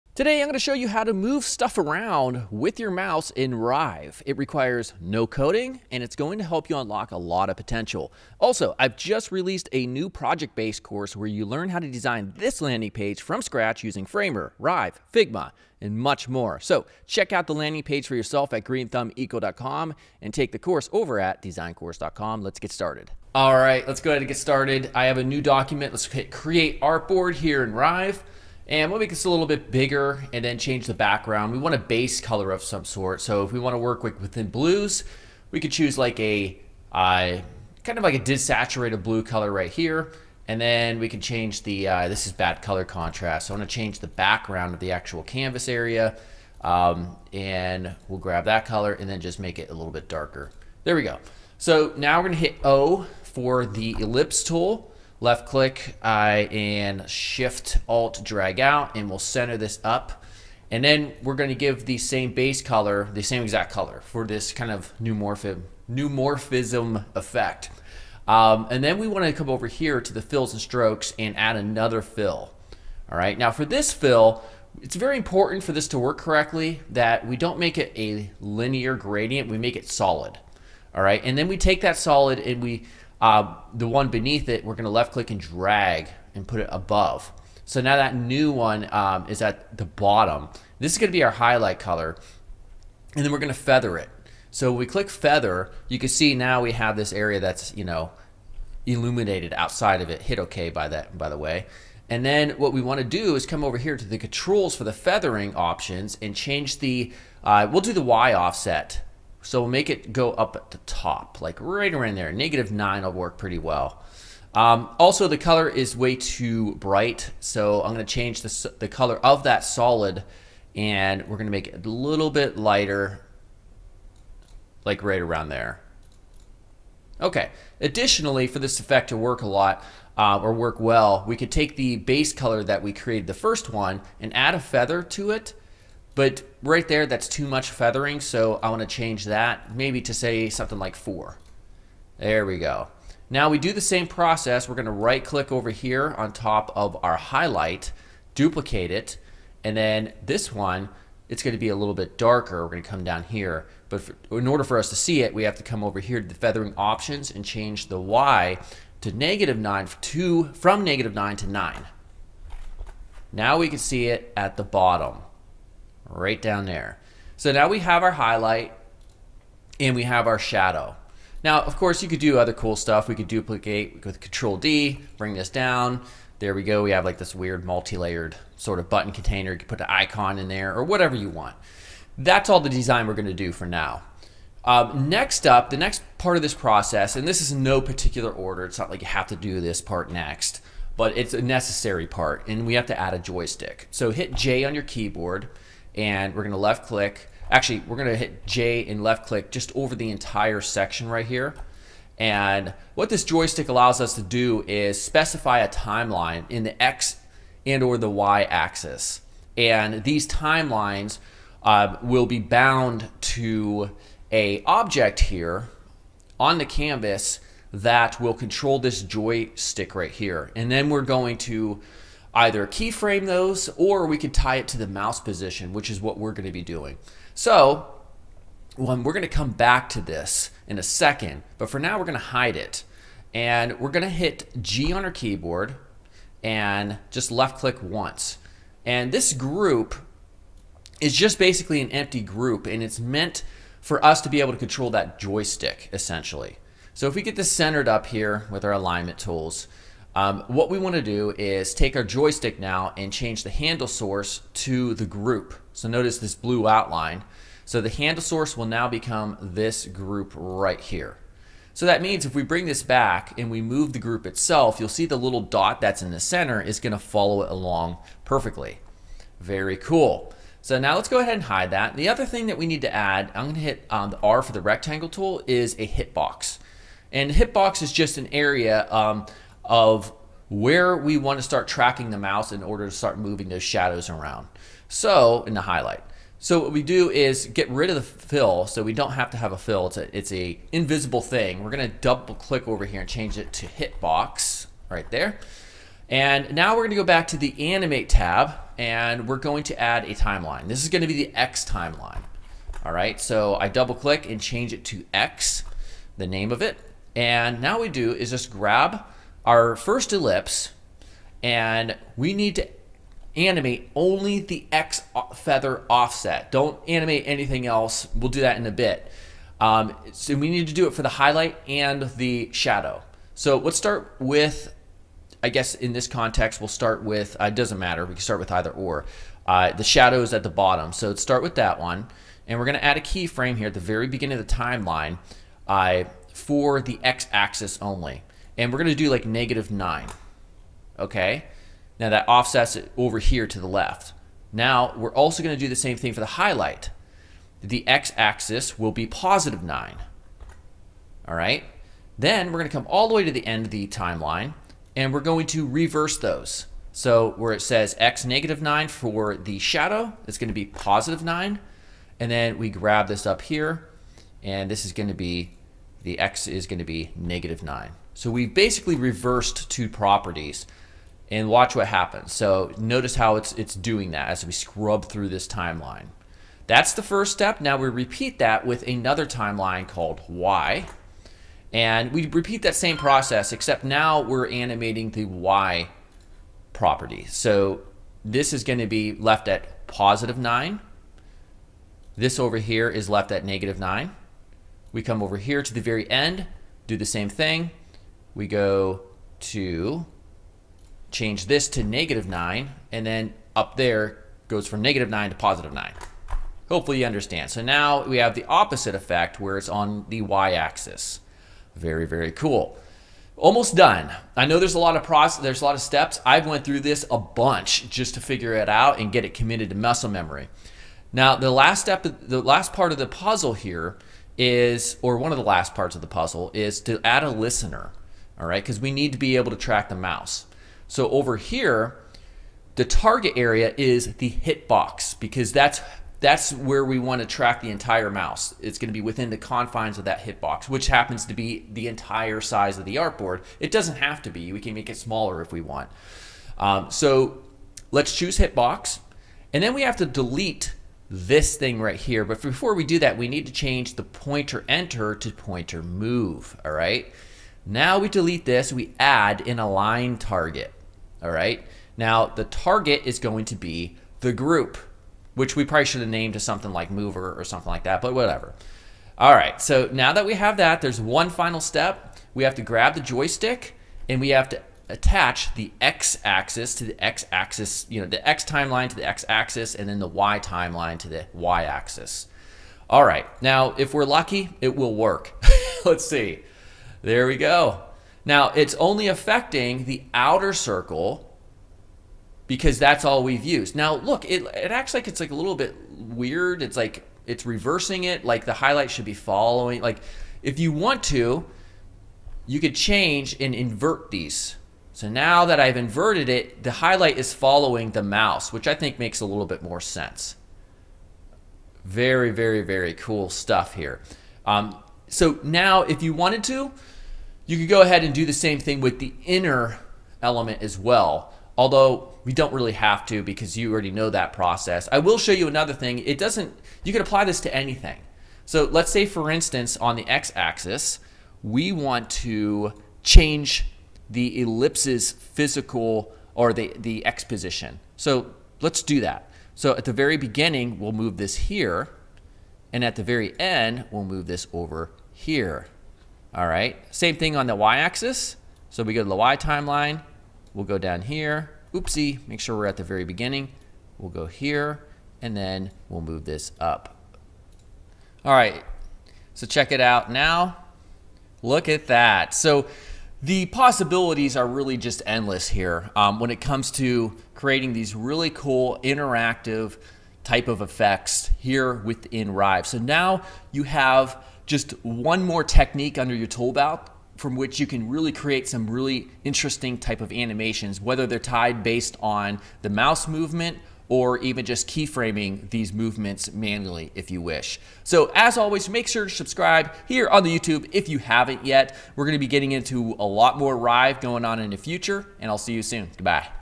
Must-know Rive Technique: Controlling the Joystick with Mouse
-- Today, I'm going to show you an extremely useful technique that allows you to animate pretty much *anything* based on the movement of the mouse within Rive.